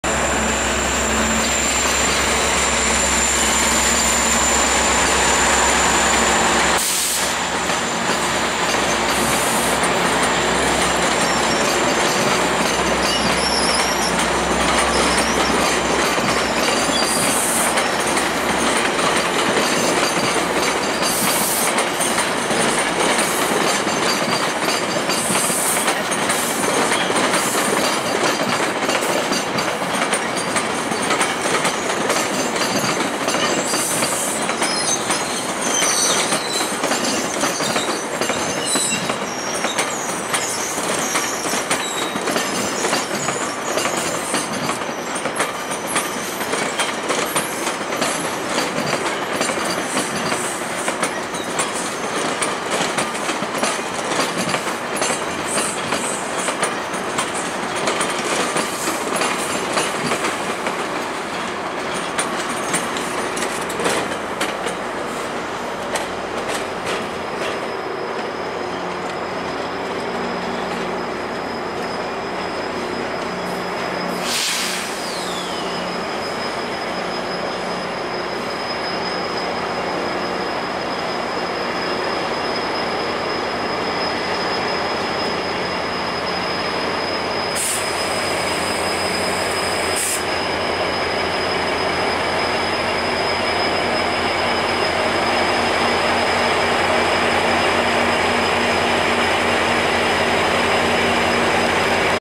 Lakeshore West GO Train at Union Station
A 12 car Lakeshore West GO train pulls into Toronto's Union Station during the morning rush hour.  The train is led by MP40 #641 with cab-car #307 bringing up the rear.